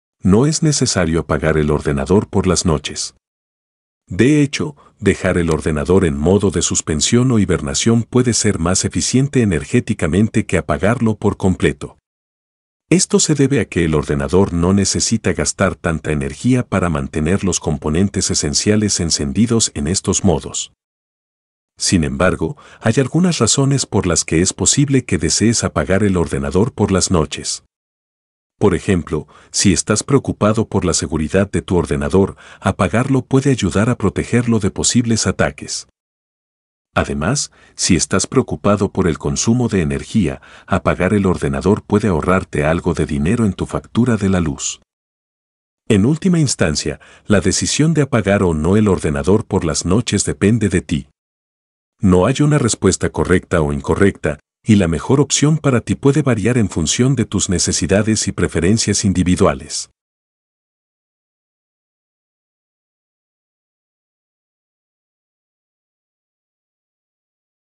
¿Es necesario apagar el ordenador por las noches?
No es necesario apagar el ordenador por las noches. De hecho, dejar el ordenador en modo de suspensión o hibernación puede ser más eficiente energéticamente que apagarlo por completo. Esto se debe a que el ordenador no necesita gastar tanta energía para mantener los componentes esenciales encendidos en estos modos.

Sin embargo, hay algunas razones por las que es posible que desees apagar el ordenador por las noches. Por ejemplo, si estás preocupado por la seguridad de tu ordenador, apagarlo puede ayudar a protegerlo de posibles ataques. Además, si estás preocupado por el consumo de energía, apagar el ordenador puede ahorrarte algo de dinero en tu factura de la luz.

En última instancia, la decisión de apagar o no el ordenador por las noches depende de ti. No hay una respuesta correcta o incorrecta, y la mejor opción para ti puede variar en función de tus necesidades y preferencias individuales.